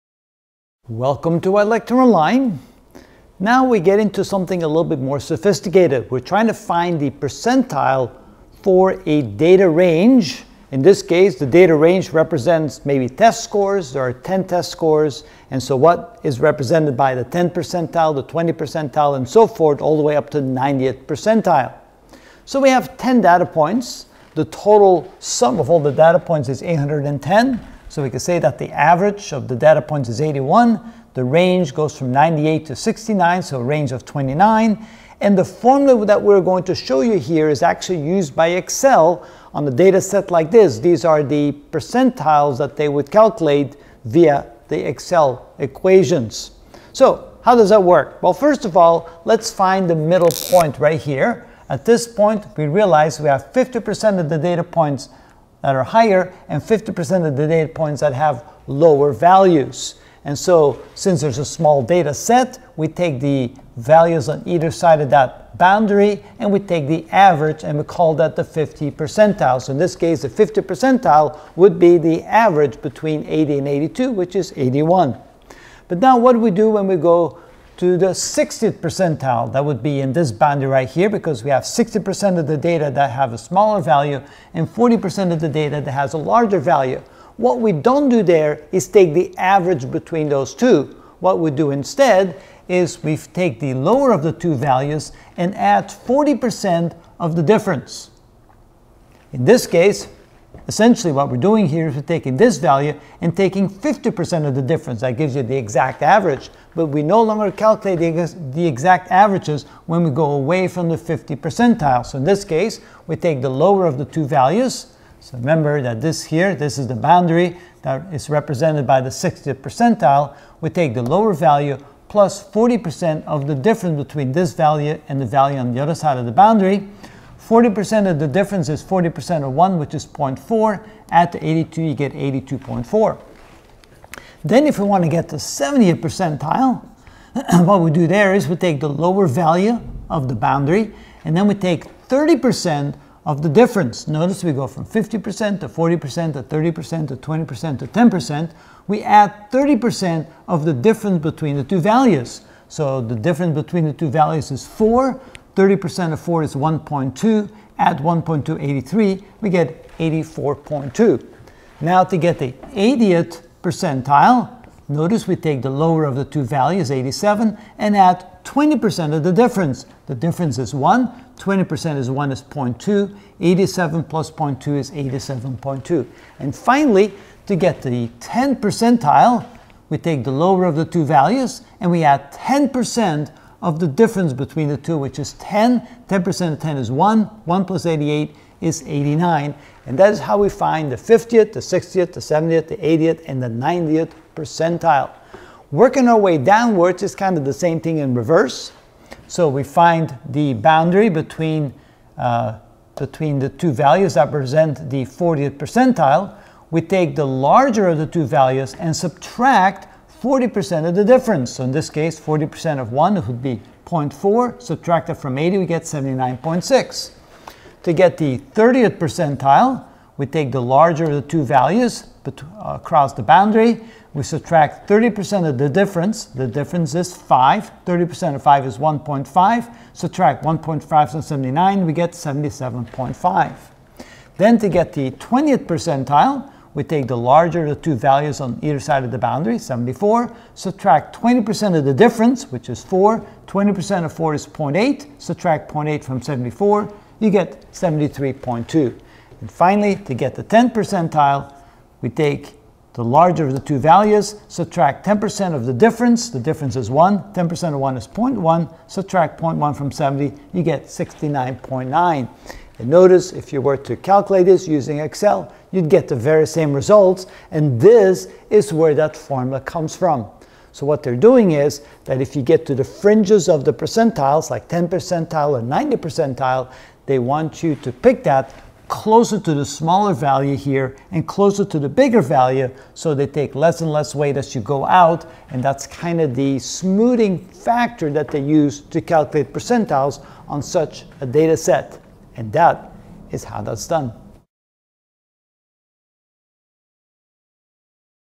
Statistic: Ch 2 Graphical Representation of Data (50 of 62) Calculating Percentile: Method 4
We will learn how to find the percentile when the data has inconsistent gaps. Method 4